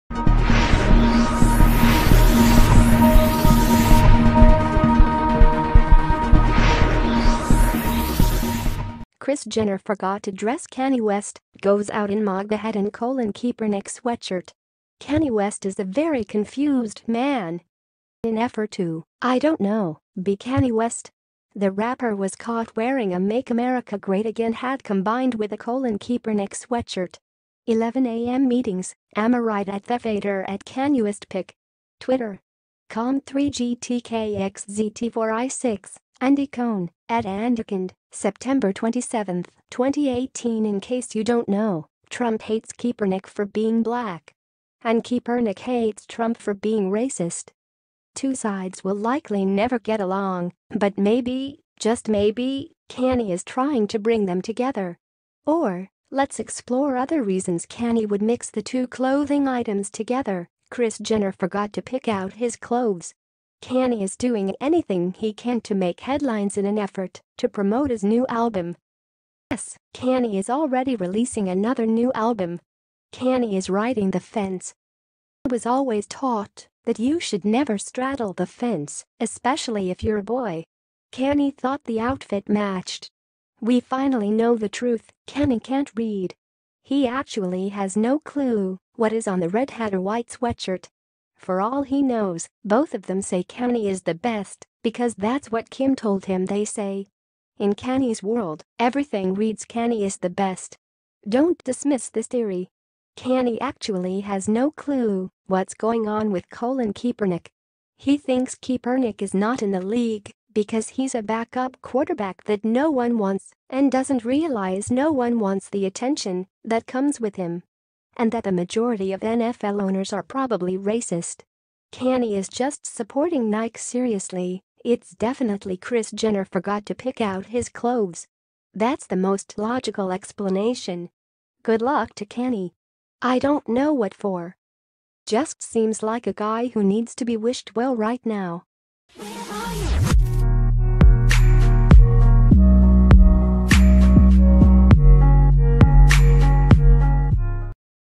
Kris Jenner Forgot to Dress Kanye West, Goes Out In MAGA Hat And Colin Kaepernick Sweatshirt
Kris Jenner Forgot to Dress Kanye West, Goes Out
In MAGA Hat And Colin Kaepernick Sweatshirt.Kanye West is a very confused man In an effort to…I don’t know, be Kanye West? The rapper was caught wearing a Make America Great Again hat combined with a Colin Kaepernick sweatshirt 11am meetings, amirite @thefader @kanyewest pic